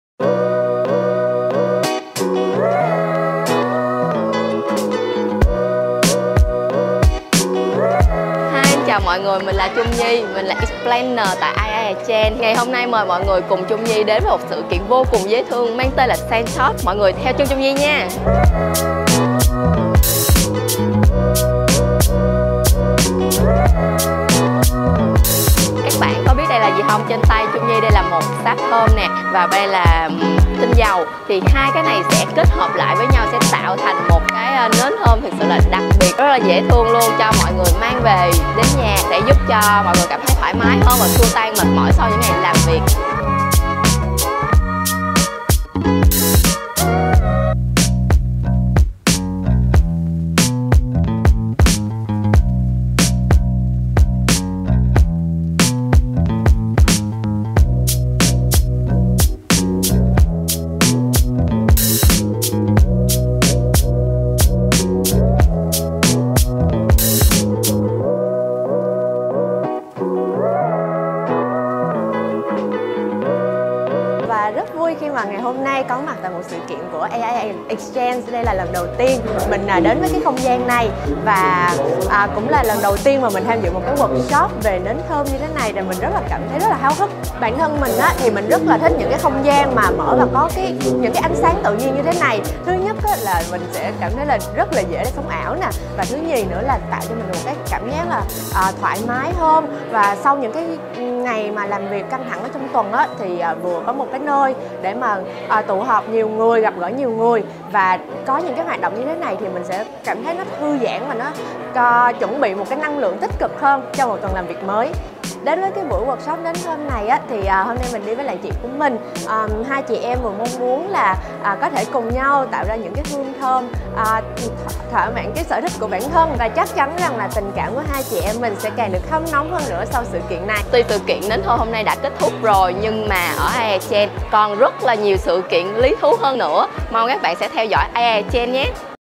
Tình chị em bền lâu hơn nhờ làm nến thơm tại AIA EXCHANGE | SCENTALK
Cùng host Chung Nhi, mời bạn tham gia chương trình làm nến thơm - một hoạt động hoàn toàn miễn phí, dành riêng đến quý khách hàng đã tham gia các chương trình bảo hiểm tại AIA exchange.

Tại đây, tình chị em của một khách hàng cũng đã được bồi đắp ngay giữa không gian bảo hiểm tuyệt đẹp này. Hãy sở hữu các giải pháp bảo hiểm và nâng tầm trải nghiệm sống cùng AIA exchange.